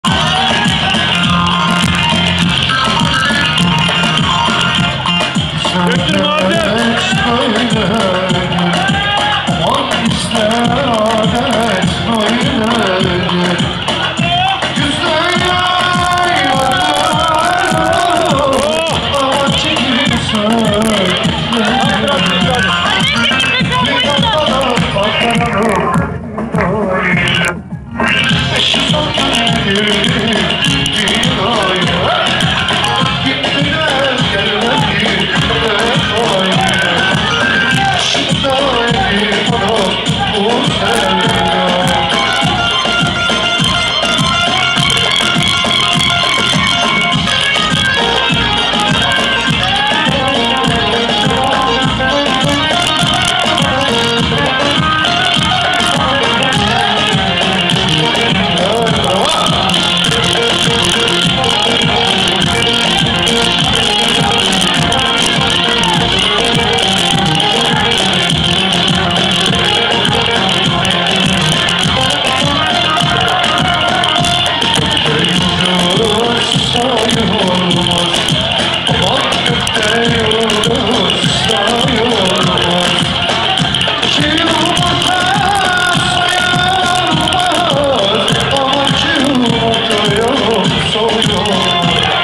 Kayseri
Kayseri Hırka Köyü
Mıstılının Torunu Muammer Ülker 'in Oğlunun Düğünü 6